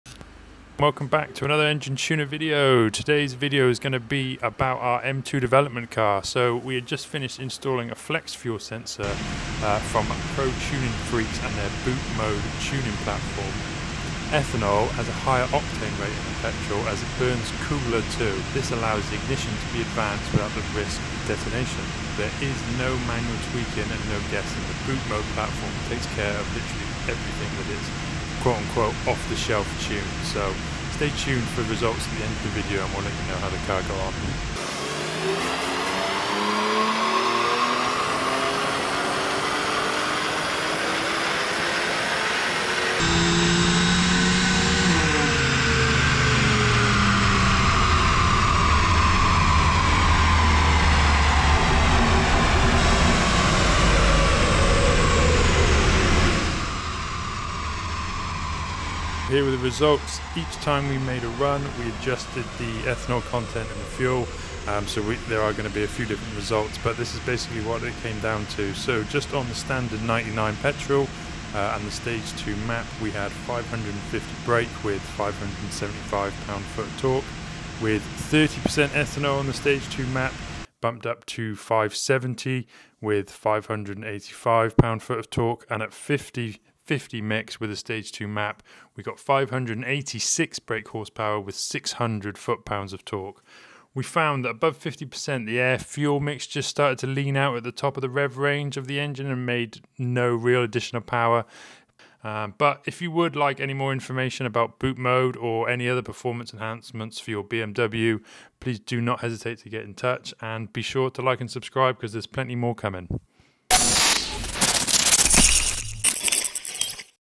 M2 Competition hits the dyno running the Bootmod3 flex fuel sensor - check out the crazy results!
Using the excellent @protuningfreaks Bootmod3 tuning platform, we installed their flex fuel sensor kit. The plug and play kit is incredibly easy to install and it requires no cutting or wiring. It can therefore be effortlessly removed if needed.

The sensor works by measuring the ethanol content of the fuel in the petrol tank. When it detects a higher concentration of ethanol, the map is optimised automatically to take advantage of thextra power potentially on offer. Ethanol has a higher octane rating than petrol and it burns cooler too. This allows the ignition to be advanced without the risk of detonation.

Importantly, there is no manual tweaking and no guessing – the Bootmod3 platform takes care of everything with its ‘off the shelf’ tune.

We made several runs, each time adjusting the ethanol content in the fuel.
If you would like more information about Bootmod3 or any other performance enhancement for your BMW, don’t hesitate to get in touch.